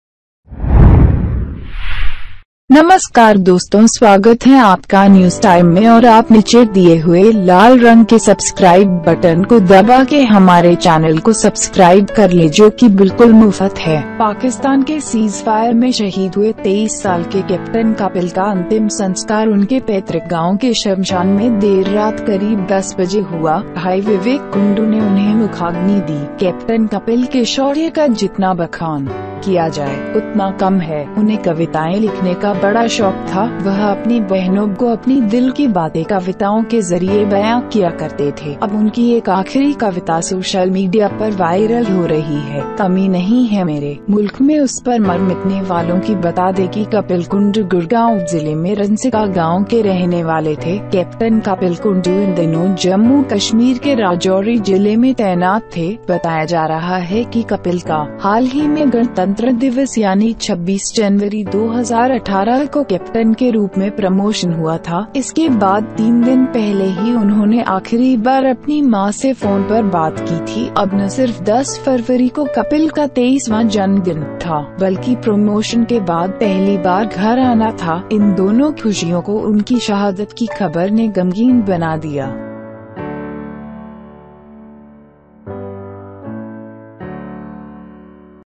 शहीद Captain Kapil Kundu ने लिखी थी यह आखिरी कविता, सोशल मीडिया पर हुई वायरल
Kapil Kundu
India army
The last rites of 23-year-old Captain Kapil, who died in Pakistan's cagefire, took place at around 10 in the crematorium of his native village late night. Bhai Vivek Kundu defeated him. As much as Captain Kapil's bravery is to be proclaimed, the less is known. He was passionate about writing poems. He used to speak to his sisters through the poems of his heart. Now one of his last poems is getting viral on social media.
There is no shortage of those who die in my country
Tell that Kapil Kundu was a resident of Ransika village in Gurgaon district. Captain Kapil Kundu was posted in Rajouri district of Jammu and Kashmir this time. It is being said that Kapil had recently been promoted as a Captain on Republic Day, i.e. 26th January, 2018. Three days after that, Mother spoke on the phone. Now, not only was Kapil's 23rd birthday on February 10, but it was for the first time to come home after the promotion. These two happinesses made the news of their martyrdom become saddening
पाकिस्तान के सीजफायर  में शहीद हुए 23 साल के कैप्टन कपिल का अंतिम संस्कार उनके पैतृक गांव के श्मशान में देर रात करीब 10 बजे हुआ।भाई विवेक कुंडू ने उन्हें मुखाग्नि दी। कैप्टन कपिल के शौर्य का जितना बखान किया जाए, उतना कम है। उन्हें कविताएं लिखने का बड़ा शौक था। वह अपनी बहनों बहनो  को अपनी दिल की बातें कविताओं के जरिए बयां किया करते थे। अब उनकी एक आखिरी कविता सोशल मीडिया पर वायरल हो रही है।
कमी नहीं है मेरे मुल्क में उस पर मर मिटने वालों की
बता दे की कपिल कुंडू गुड़गांव जिले में रनसिका गांव के रहने वाले थे। कैप्टन कपिल कुंडू इन दिनों जम्मू- कश्मीर के राजौरी जिले में तैनात थे।बताया जा रहा है कि कपिल का हाल ही में गणतंत्र दिवस यानि 26 जनवरी 2018 को कैप्टन के रूप में प्रमोशन हुआ था।इसके बाद तीन दिन पहले ही उन्होेंने आखिरी बार अपनी मां से फोन पर बात की थी। अब न सिर्फ 10 फरवरी को कपिल का 23वां जन्मदिन था, बल्कि प्रोमोशन के बाद पहली बार घर आना था। इन दोनों खुशियों को उनकी शहादत की खबर ने गमगीन बना दिया।